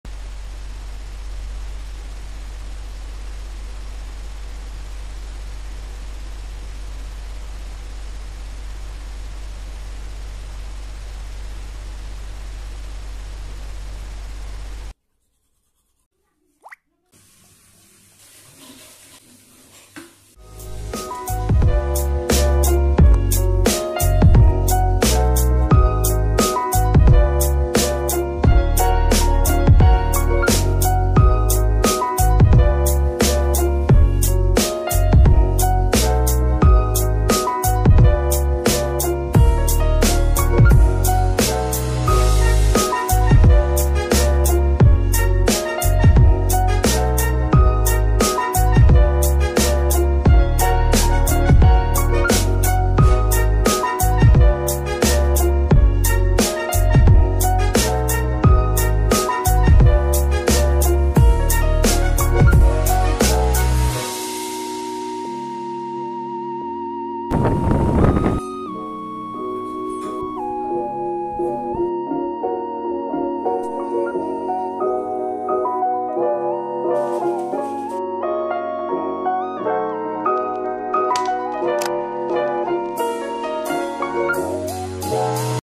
Day 019 | a day in the field | backing on the track
Day 019 | a day in the field | backing on the track | lifting weights as a footballer.